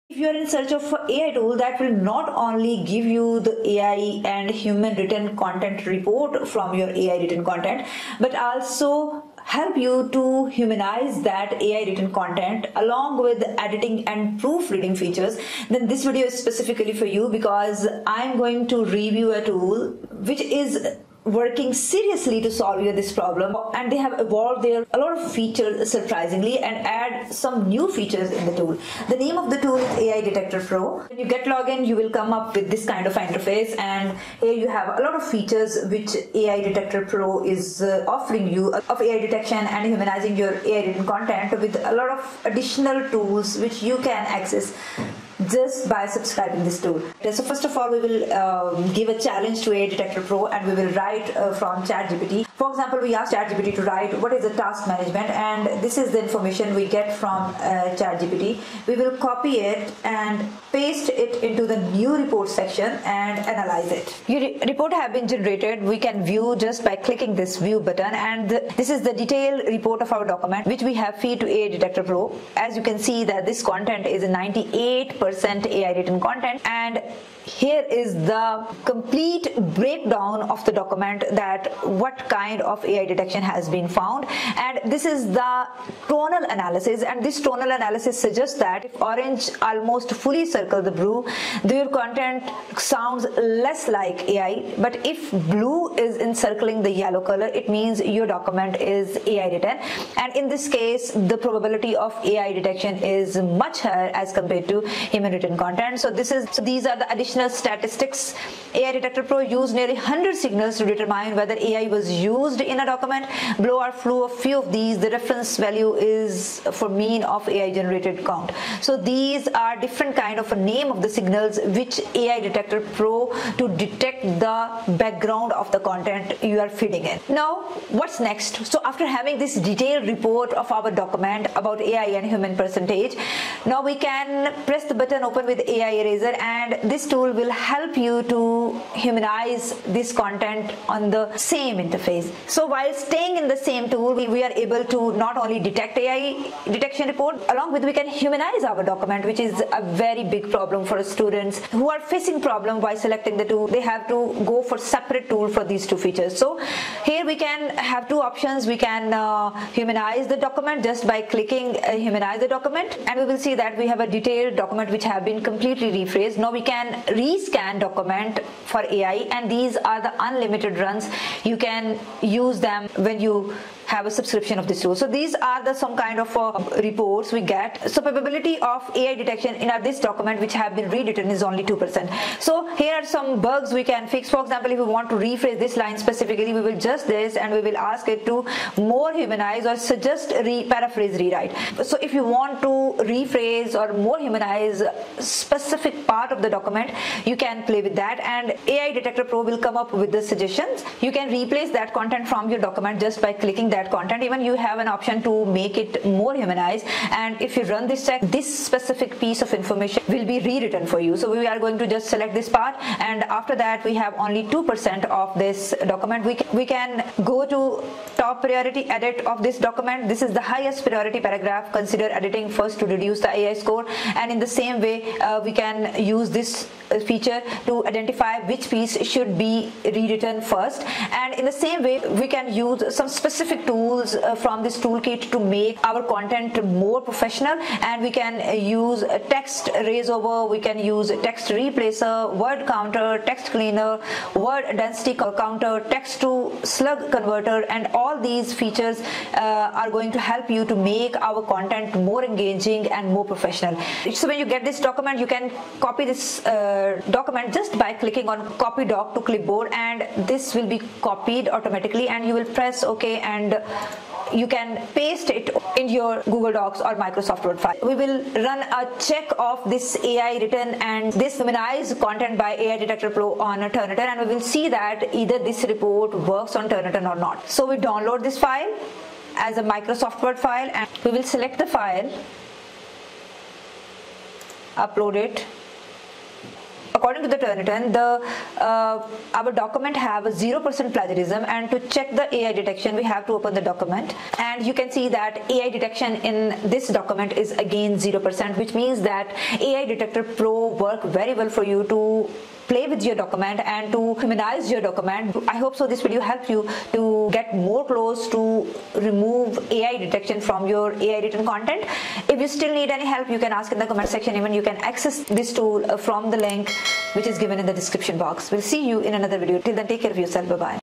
Ultimate Tool for Ai Detection & Removing form AI-generated content | AI Detector Pro
AI Detection and AI Removal With The Same Tool | AI Detector Pro |By Pass AI
Timesmap

0:00 Introduction
00:47 Check the Ai detection of Chatgpt written content
02:07 Humanize AI written content With AI Detector pro
03: 5 Editing and proof Reading of Research Document
04: 28 Ai detection and Plagiarism check through Turnitin

Watch this video to know how to interpret the Turnitin originality report - Guidance for students